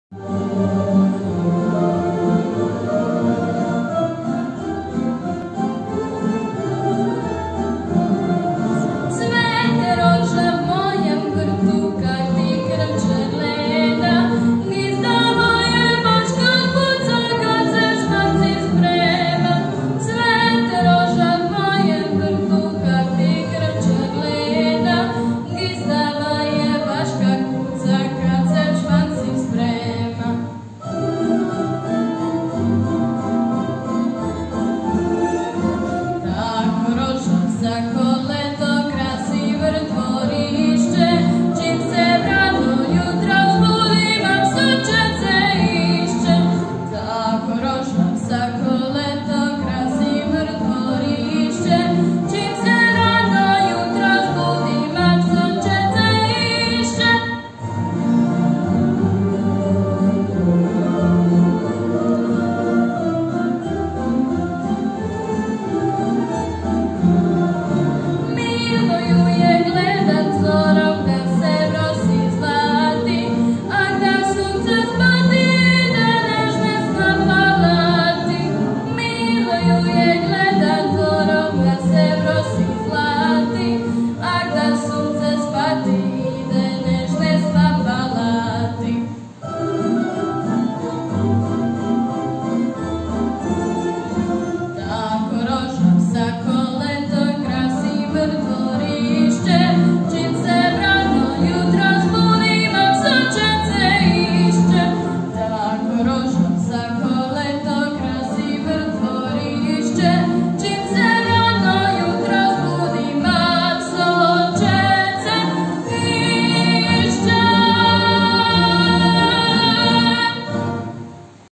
Ana Matošević - proba za Zlatar 2021
Ana Matošević 5c razred OŠ Josipa Račića - proba za Festiva kajkavske popevke u Zlataru